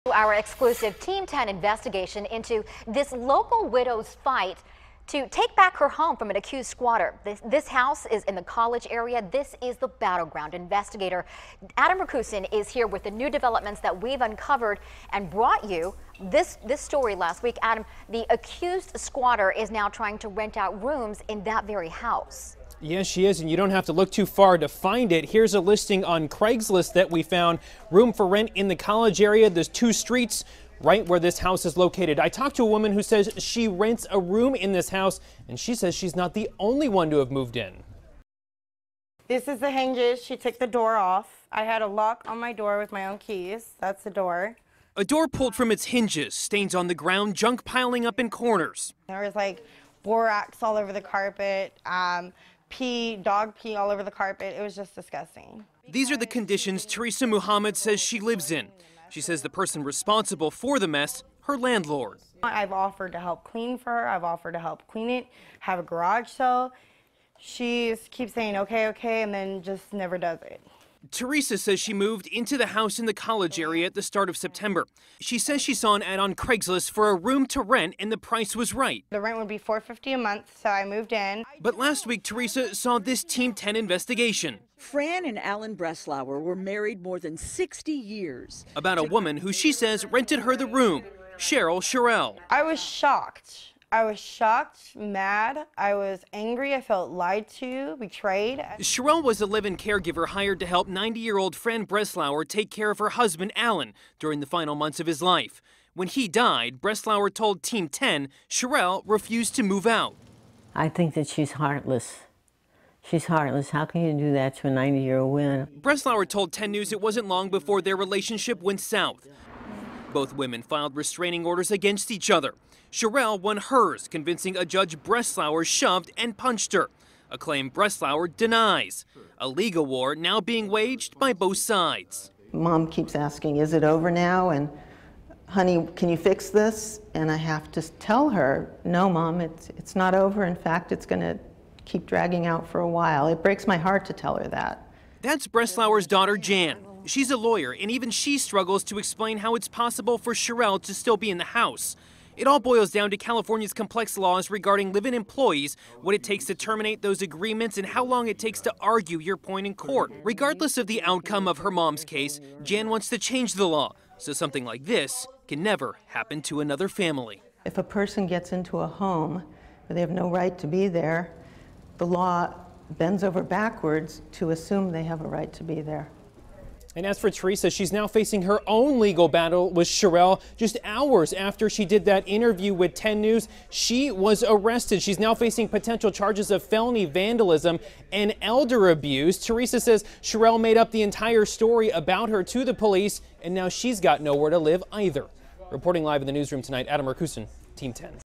Accused squatter renting out rooms out at College Area home
A woman who moved into a home that is the subject of a legal battle told Team 10 she felt her landlord lied to her.